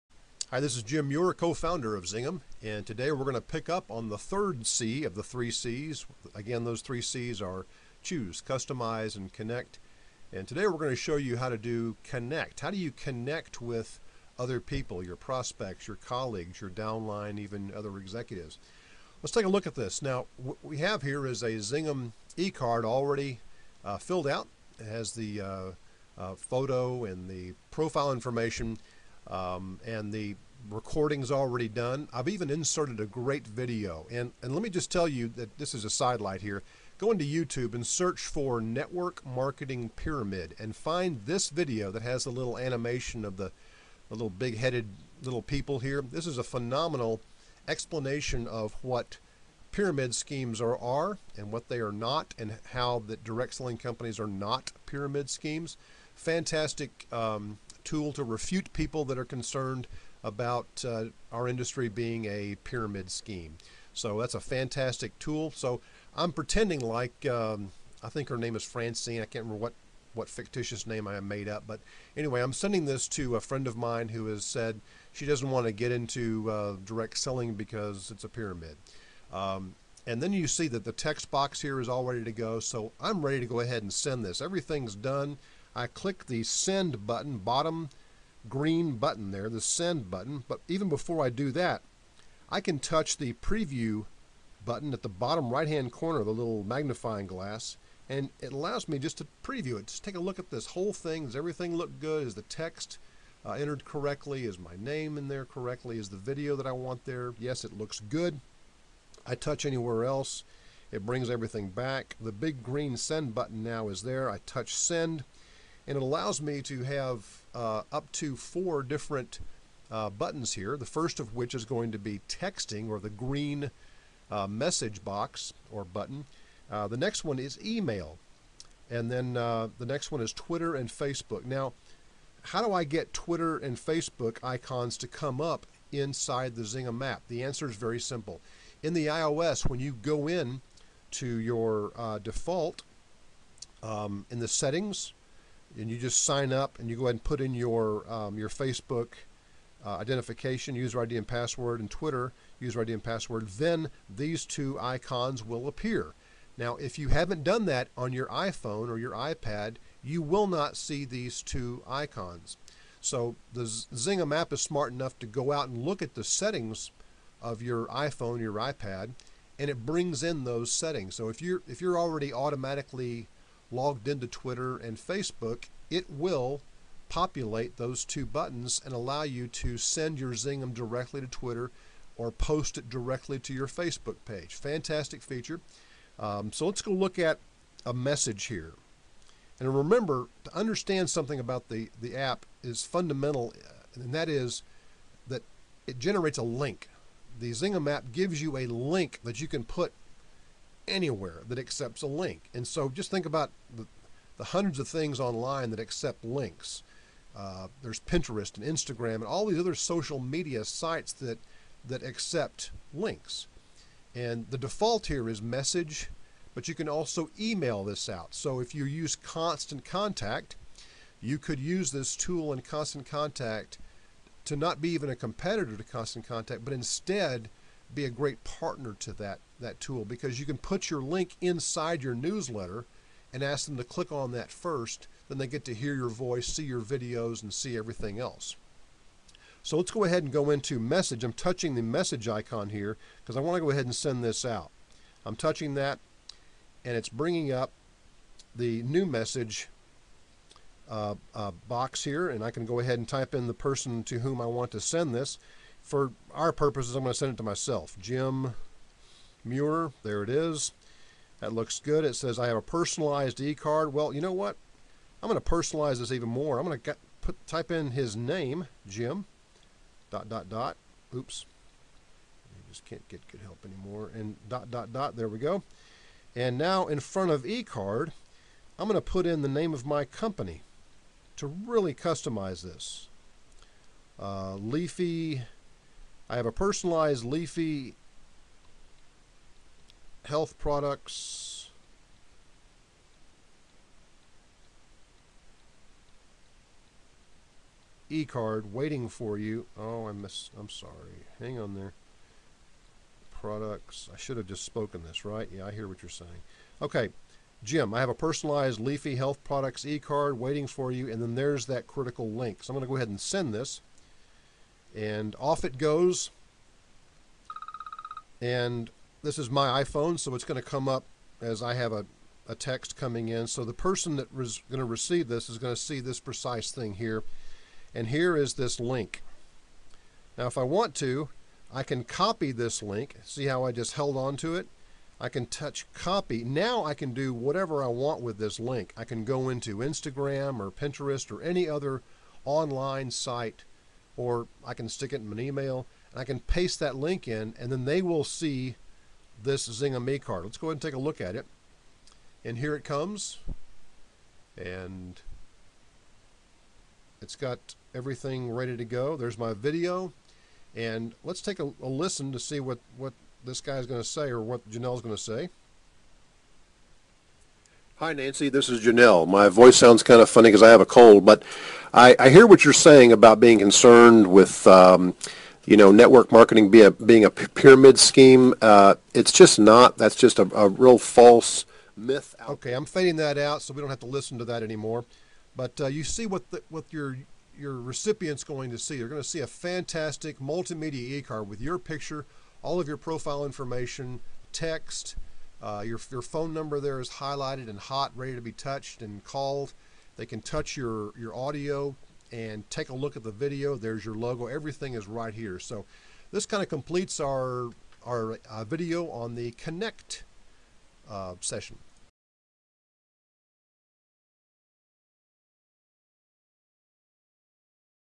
ZINGem Step 3: Connect
This video shows you the many ways in which you can leverage ZINGem to connect with others (text, email, Twitter, Facebook, etc.)